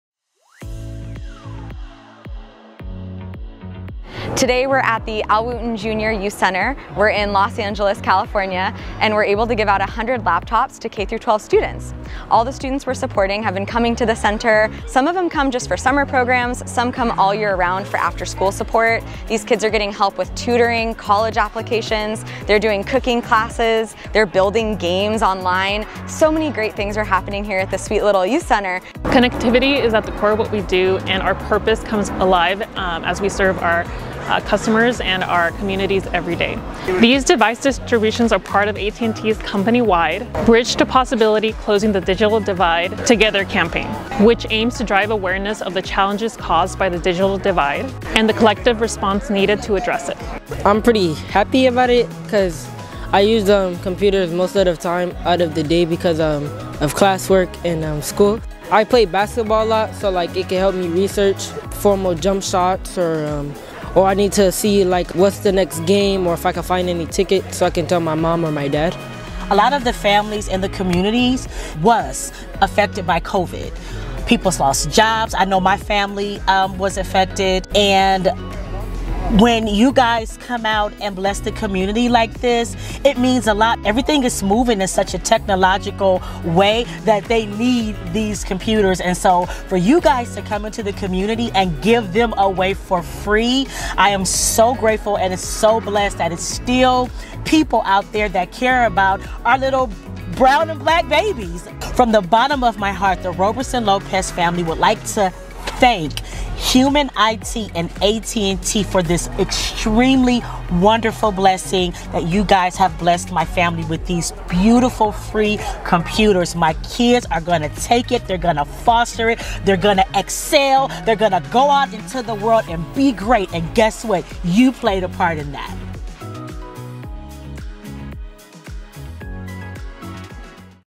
Closing the Digital Divide with AT&T's Bridge to Possibility Campaign
Another day, another 100 students in Los Angeles who are now equipped with the digital access they need to thrive in and out of the classroom.

These are the types of impacts that our partners like AT&T make possible when they decide to invest in their communities.

With our partners' support, we're going to create a world where everyone has equal access to the digital tools, connectivity, and support they need to unlock their full potential.